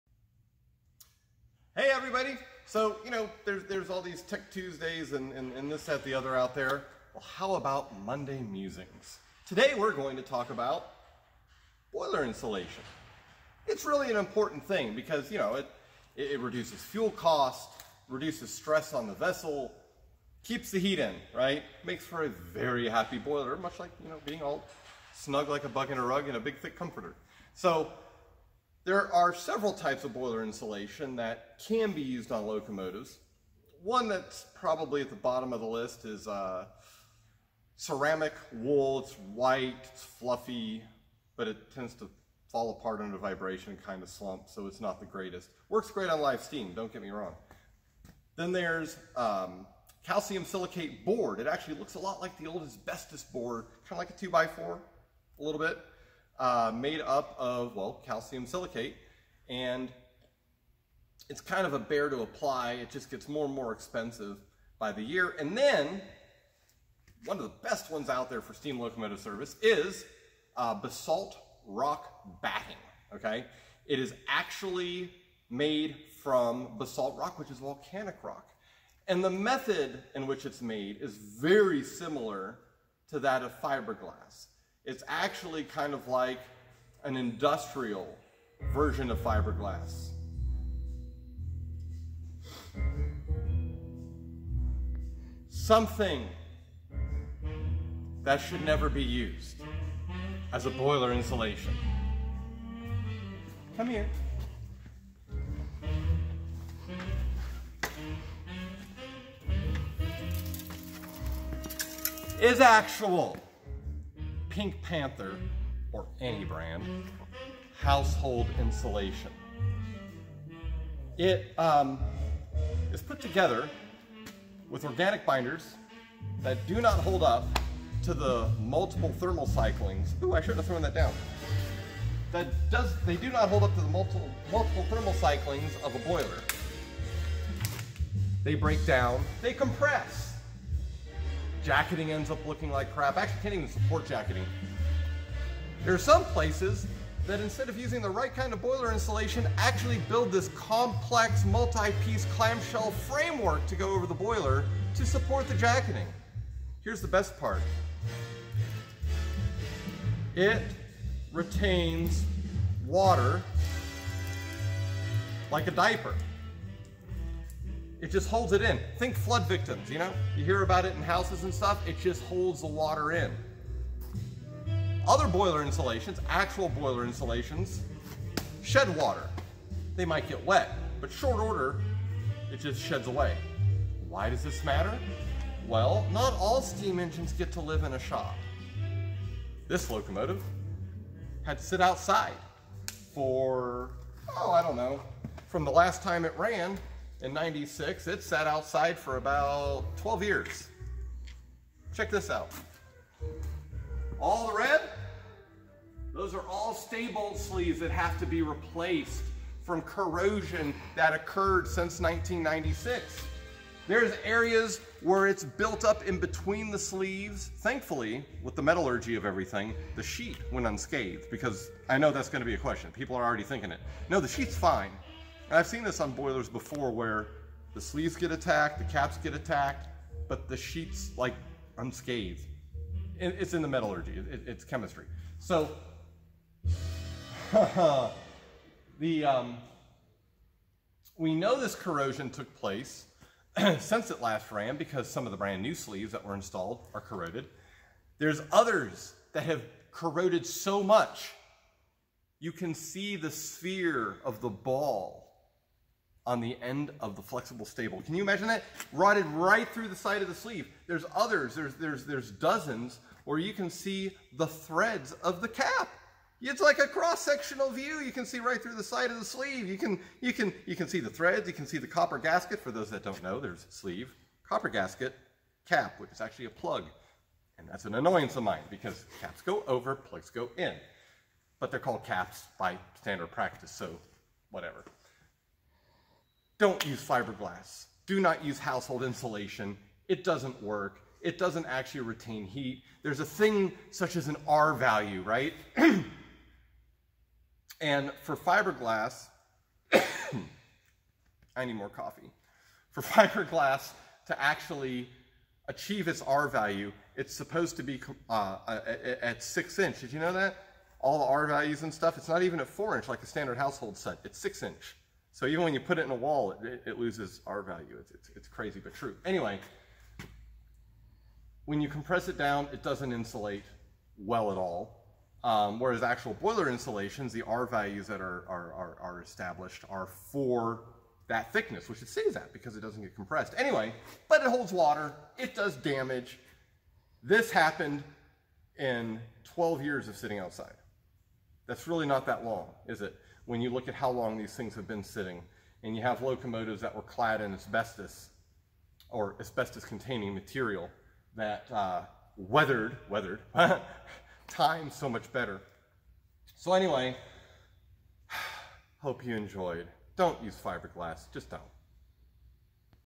Monday Musings: Boiler Insulation
Standard practices, both of the historical steam railroad sort and modern industrial, exist for very good reasons. When they are followed one not only sees superior results, but often times we see long term cost savings.